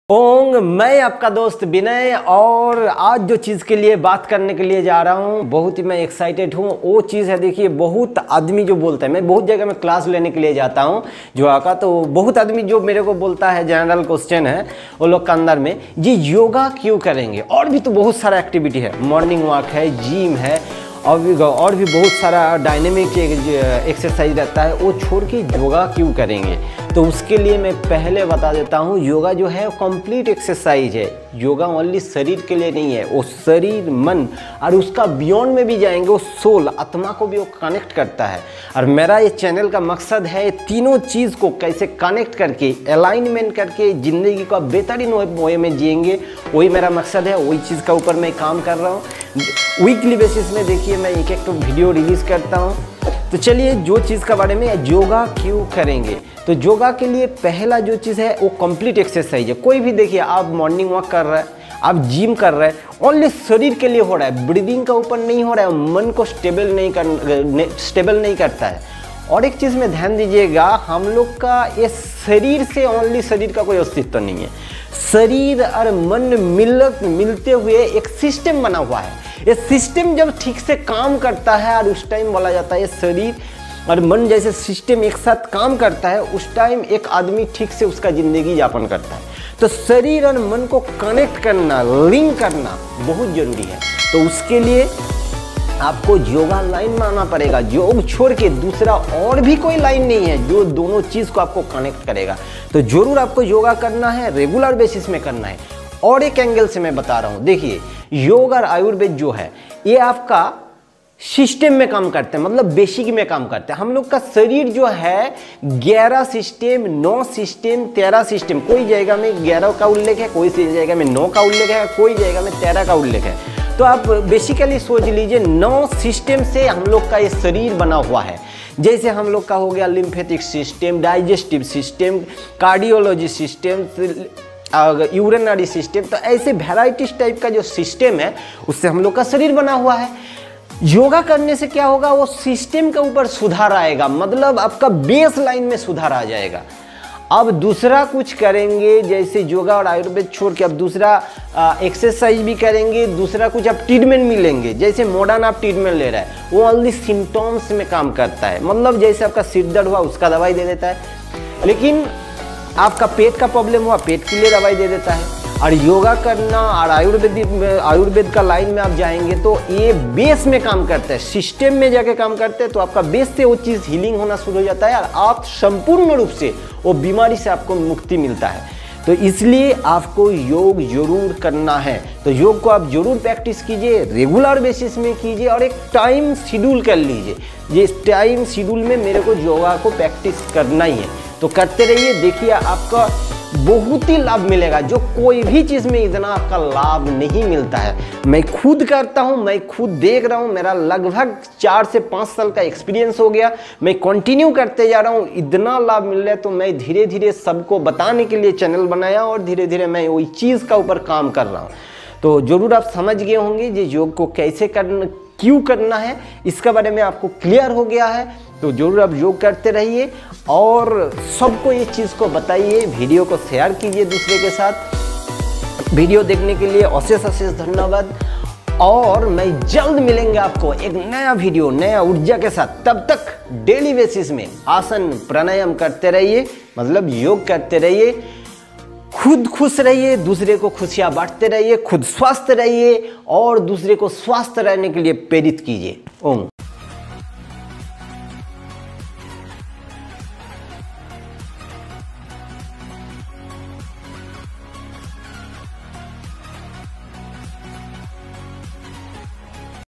Yoga is important than other exercises || Hindi || Binoy
In this video I am explained, why you will practice yoga in regular basic and shifted in yogic management

To know more about yoga get in touch with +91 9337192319 (via Call from Monday to Saturday 8 PM to 9 PM)
We would love to receive your Questions, Feedbacks & Suggestions in the Comment Section. Thank You.

Background Music
Track Info